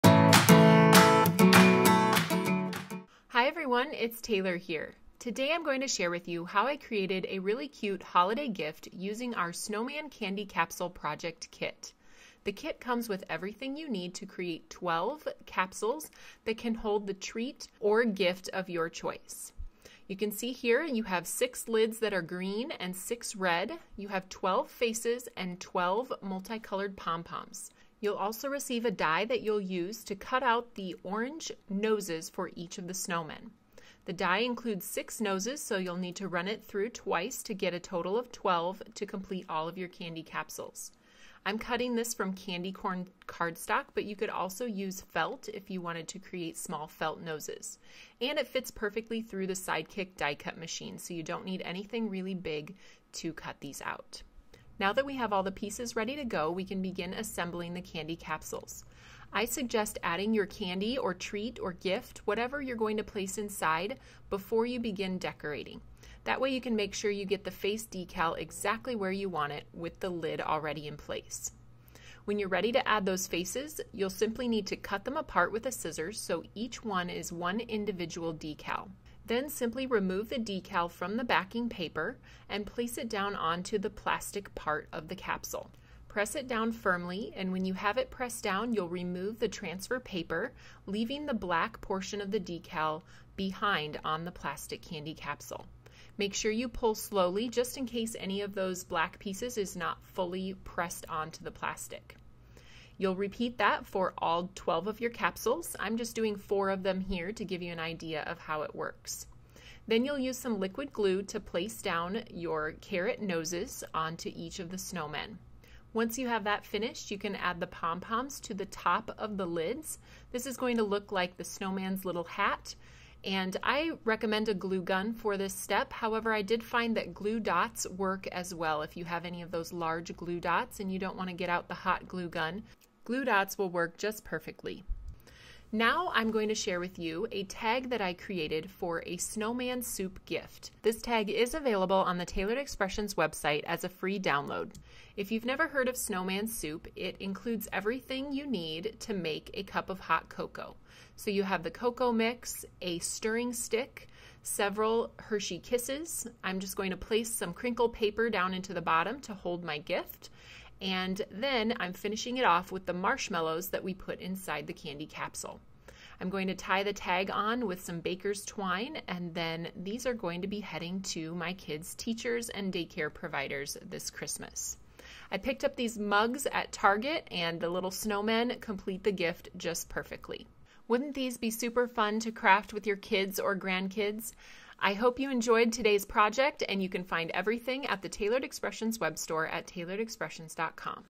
Snowmen Candy Capsules | DIY Holiday Treats | Taylored Expressions
Earlier this fall we launched some adorable Pumpkin Candy Capsules; we've now launched Snowmen Candy Capsules for winter! Taylor is going to show you how to create a sweet treat for your friends, family, neighbors or coworkers! You can fill these capsules with marshmallows or other festive candies to give out this holiday season.